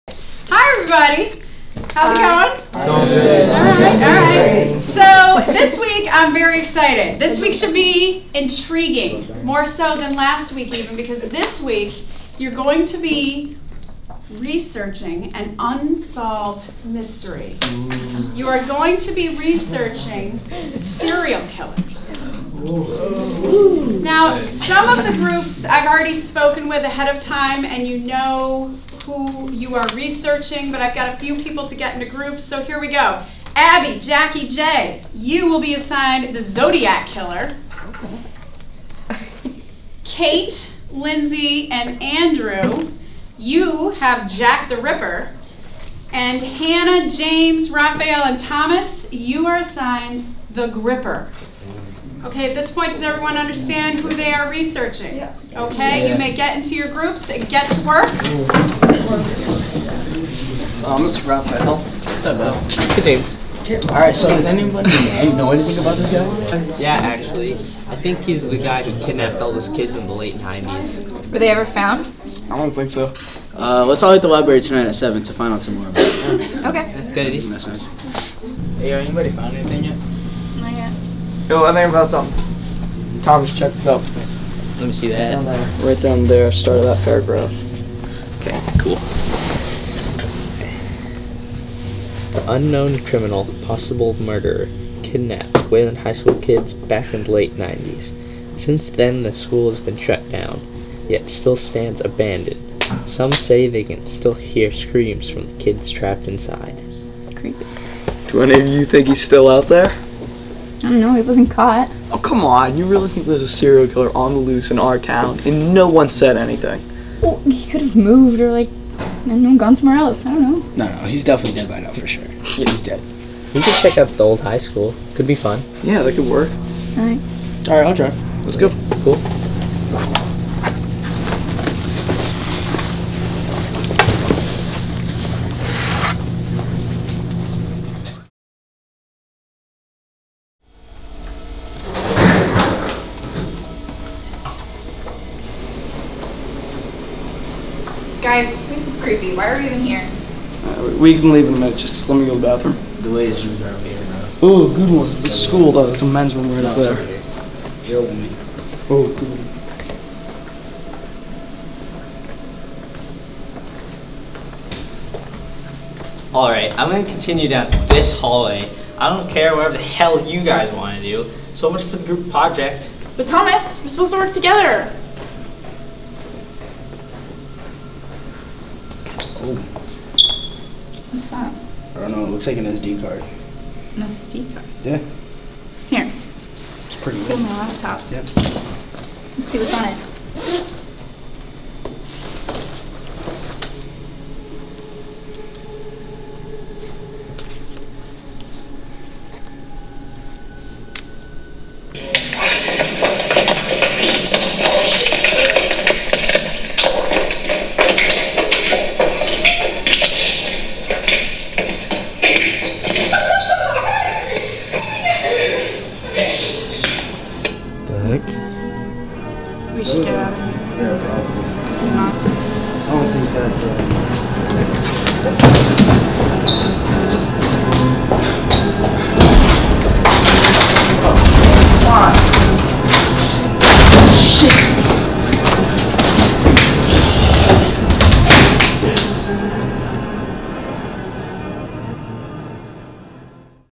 Quiet on the set!
Intrepid WHS teens are caught in a web of terror!!! In this video clip filmed right here at WFPL!